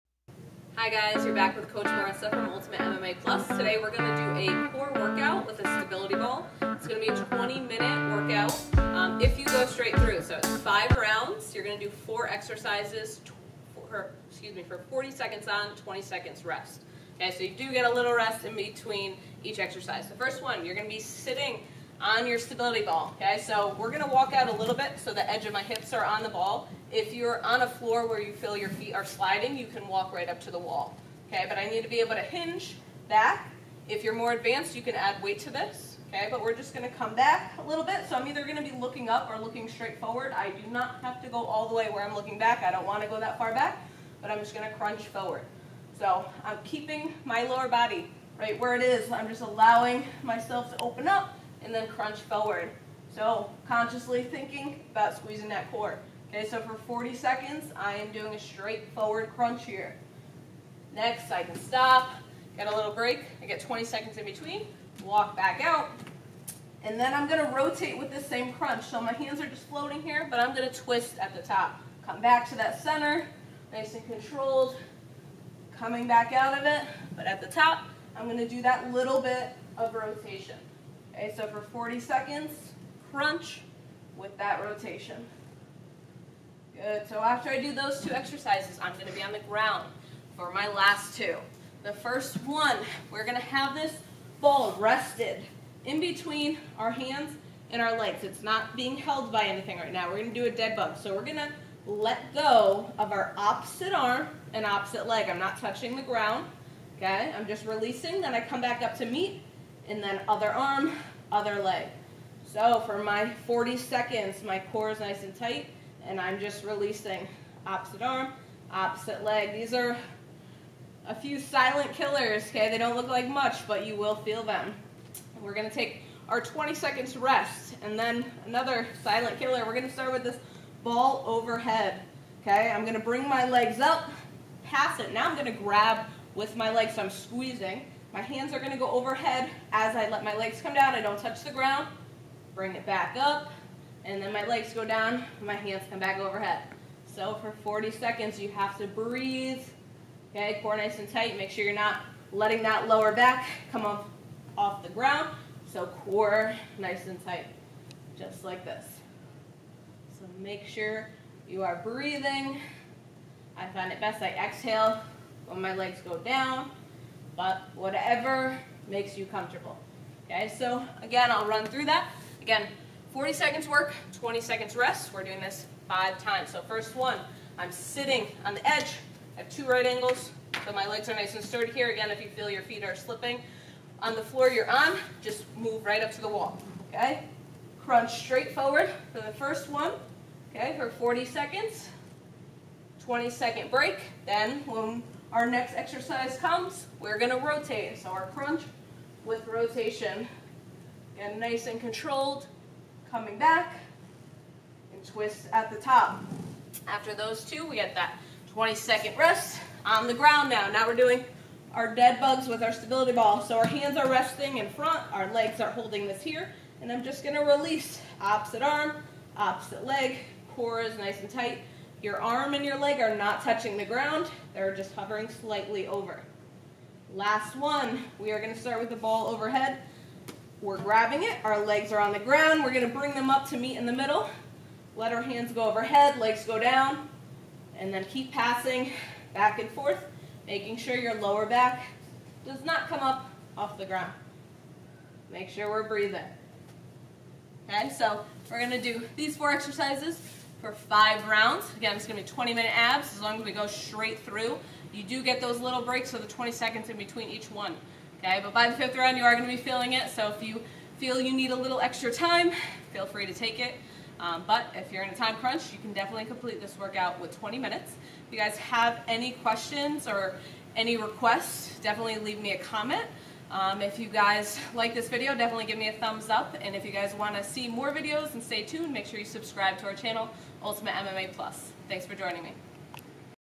20 Minute Stability Ball Core Workout: Strengthen and Tone Abs
In today's video, Coach Marisa will show you a great 20 Minute Stability ball core workout. Below is the full workout!

20 Minute Stability Ball Core Workout
5 Rounds of 4 Exercises
40 seconds of work, 20 seconds of rest in between each exercise

- Stability Ball Seated Crunch
- Stability Ball Seated Crunch with Rotation
- Stability Ball Alternating Opposite Arm/Leg Release
- Stability Ball Hand to Foot Pass Leg Raise

Option to increase seated crunch difficulty by adding weight for extra resistance.